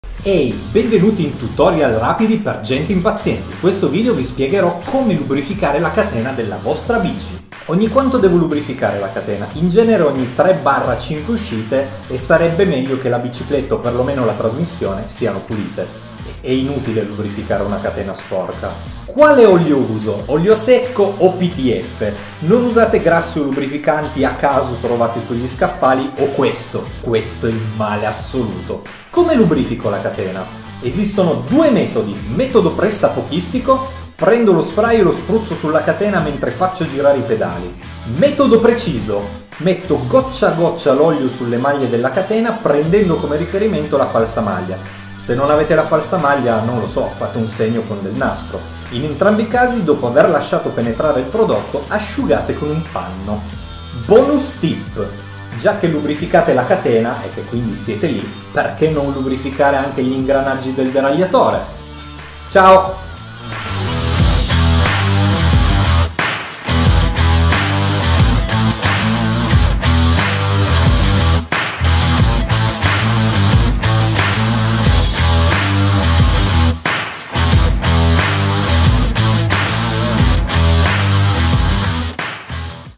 Come lubrificare male la catena della bicicletta
Sono l'unico a non essere mai arrivato in fondo ad un tutorial di aranzulla? Sono l'unico che vorrebbe dei tutorial che arrivino dritti al punto?
Per i non pignoli nasce quindi la serie "TUTORIAL RAPIDI PER GENTE IMPAZIENTE".

★★★LINK PRODOTTI★★★
FINISH LINE
WD40 SPRAY